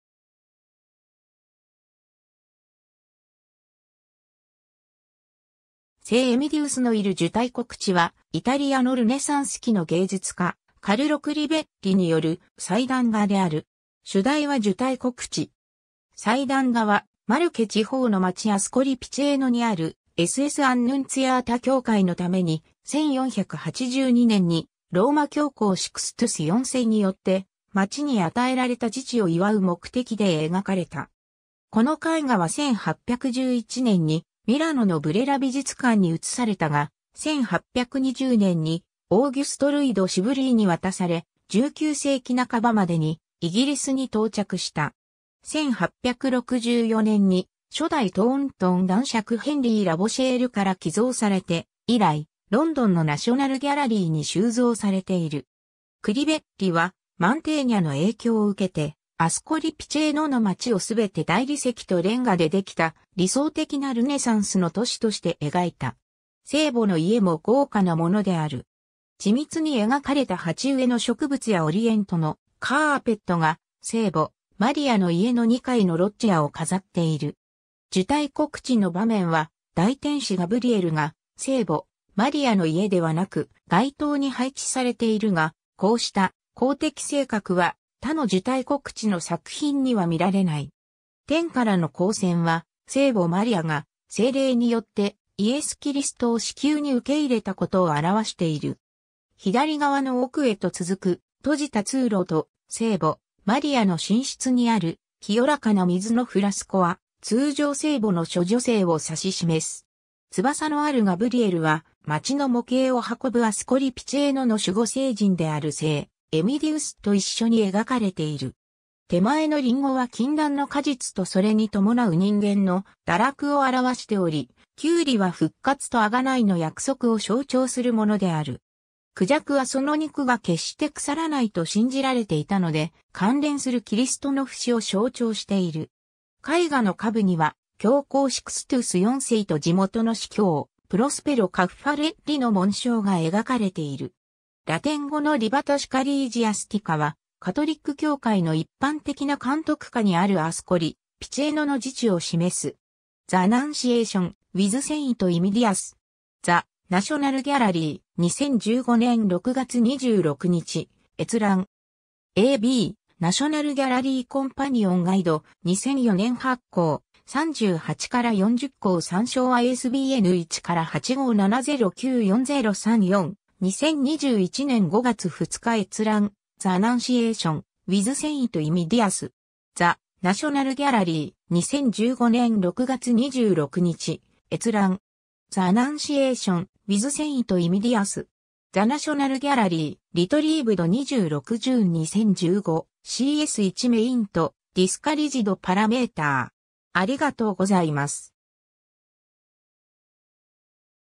聖エミディウスのいる受胎告知
カルロ・クリヴェッリの作品
ロンドン・ナショナル・ギャラリーの所蔵品
『聖エミディウスのいる受胎告知』（せいエミディウスのいるじゅたいこくち, 伊: Annunciazione di Ascoli, 英: The Annunciation, with Saint Emidius）は、イタリアのルネサンス期の芸術家カルロ・クリヴェッリによる祭壇画である。
主題は受胎告知。
祭壇画はマルケ地方の町アスコリ・ピチェーノにあるSSアンヌンツィアータ (受胎告知) 教会のために、1482年にローマ教皇シクストゥス4世によって町に与えられた自治を祝う目的で描かれた。
この絵画は1811年にミラノのブレラ美術館に移されたが、1820年にオーギュスト＝ルイ・ド・シヴリーに渡され、19世紀半ばまでにイギリスに到着した。
1864年に初代トーントン男爵ヘンリー・ラボシェールから寄贈されて以来、ロンドンのナショナル・ギャラリーに収蔵されている 。
 クリヴェッリはマンテーニャの影響を受けて、アスコリ・ピチェーノの町をすべて大理石とレンガでできた理想的なルネサンスの都市として描いた。
聖母の家も豪華なものである。
緻密に描かれた鉢植えの植物やオリエントのカーペットが聖母マリアの家のニ階のロッジアを飾っている。
 受胎告知の場面は、大天使ガブリエルが聖母マリアの家ではなく街頭に配置されているが、こうした公的性格は他の受胎告知の作品には見られない。
天からの光線は、聖母マリアが聖霊によってイエス・キリストを子宮に受け入れたことを表している。
左側の奥へと続く閉じた通路と聖母マリアの寝室にある清らかな水のフラスコは、通常聖母の処女性を指し示す。
翼のあるガブリエルは、町の模型を運ぶアスコリ・ピチェーノの守護聖人である聖エミディウスと一緒に描かれている。
手前のリンゴは禁断の果実とそれに伴う人間の堕落を表しており、キューリは復活と贖いの約束を象徴するものである。
孔雀はその肉が決して腐らないと信じられていたので、関連するキリストの不死を象徴している。
 絵画の下部には、教皇シクストゥス4世と地元の司教プロスペロ・カッファレッリの紋章が描かれている 。
ラテン語の libertasecclesiastica（教会の自由）は、カトリック教会の一般的な監督下にあるアスコリ・ピチェーノの自治を示す。
 ^ “The Annunciation, with Saint Emidius”. The National Gallery. 2015年6月26日閲覧。
 ^ a b 『ナショナル・ギャラリーコンパニオン・ガイド』、2004年発行、38-40項参照　ISBN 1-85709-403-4、2021年5月2日閲覧 ^ “The Annunciation, with Saint Emidius”. The National Gallery. 2015年6月26日閲覧。
"The Annunciation, with Saint Emidius". The National Gallery. Retrieved 26 June 2015. CS1 maint: discouraged parameter (link)